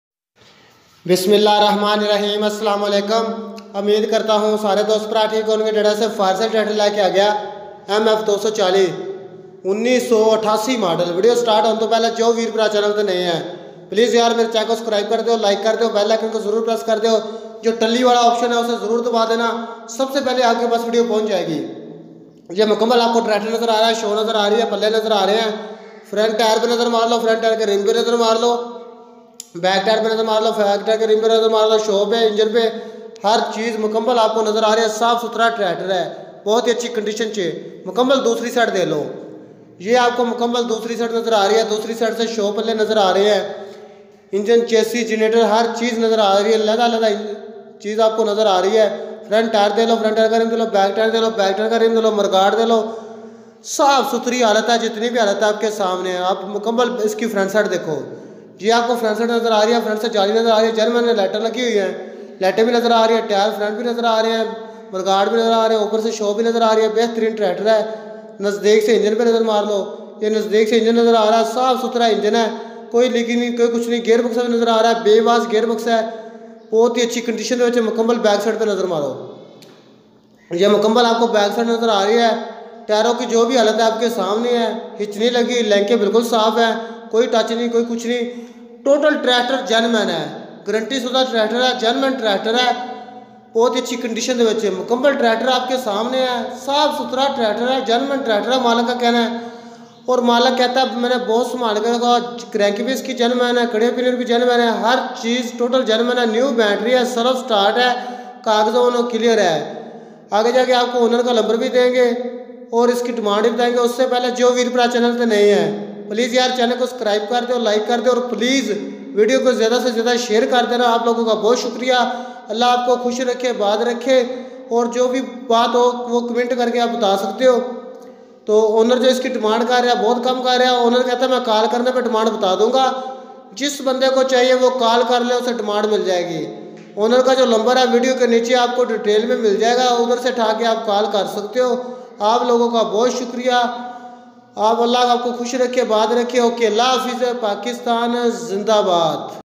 MF 240 tractor for sale 1988 model MF 265 MF 135 MF 375 MF 260
MF 240 tractor for sale 1988 model good condition used to store new battery self start